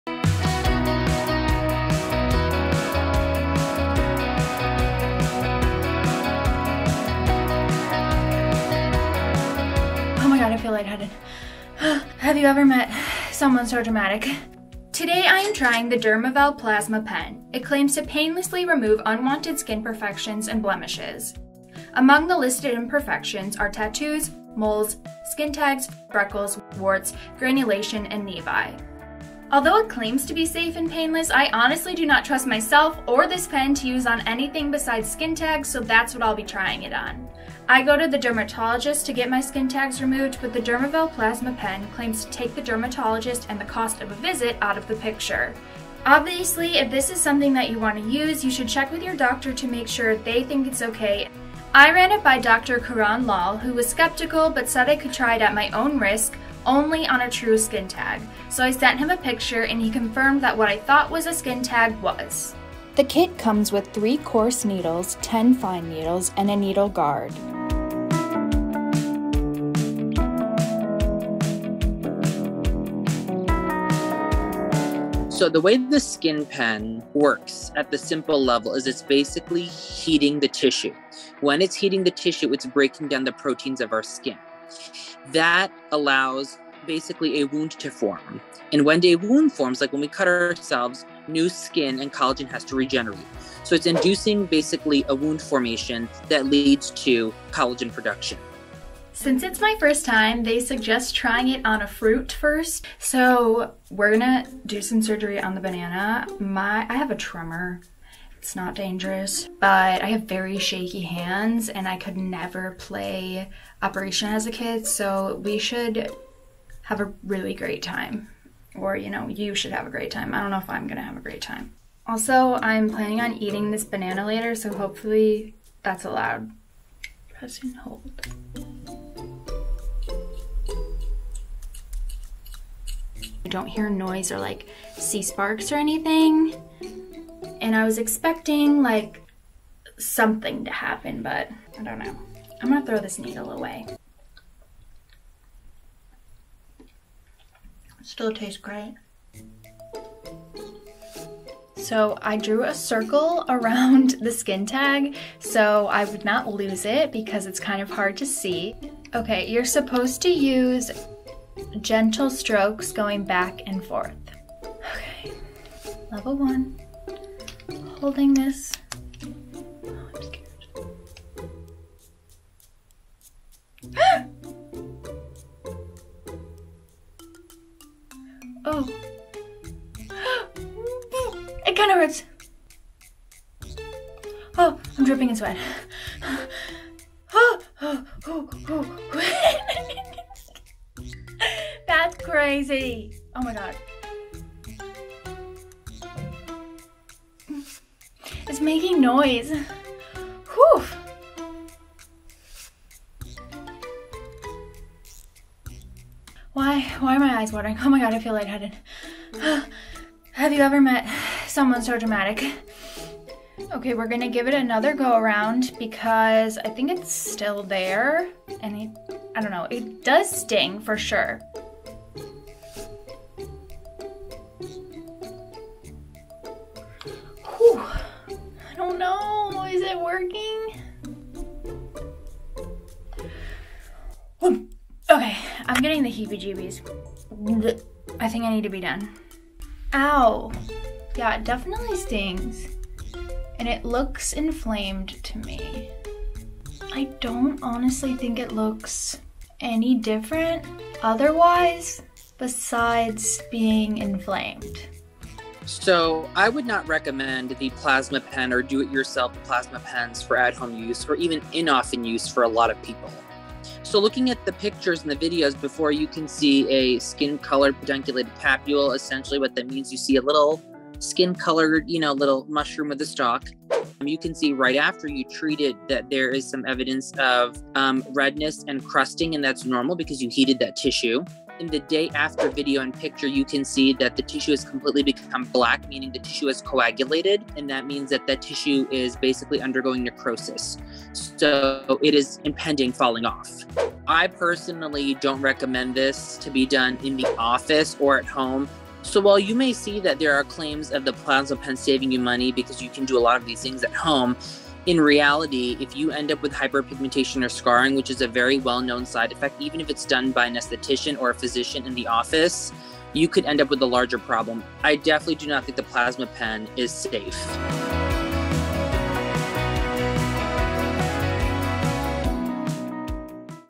Heated Needle Tool Claims To Remove Skin Tags And Moles At Home | The Skin Fix | Beauty Insider
Usually to get skin tags, moles, tattoos, and other skin bumps and marks removed, you have to go to the dermatologist. But according to Dermavel, all you need to do is buy its plasma pen. Dermavel says the pen painlessly removes unwanted skin imperfections and blemishes at home. It comes with fine and coarse needles, depending on what you are looking to remove. Insider producer Celia Skvaril decided to try it on a skin tag, but ran it past a dermatologist first. Dr. Karan Lal was skeptical, but said Celia could try it at her own risk as long as she sent over a photo to make sure she actually had a true skin tag. After trying the pen, Celia ran past the results by Lal, who said although technically the device worked, he doesn't think it's safe.

Check out the Dermavel plasma pen here:
Dermavel Plasma Pen, $99

Check out the microscope here:
Celestron Deluxe Handheld Digital Microscope, $99.95